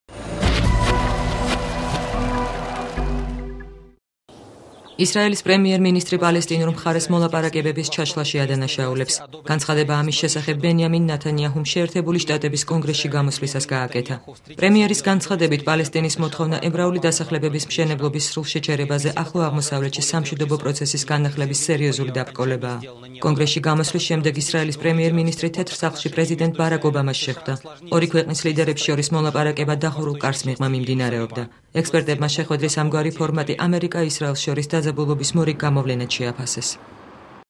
24/03/2010
ისრაელის პრემიერმინისტრი აშშს პრეზიდენტს შეხვდა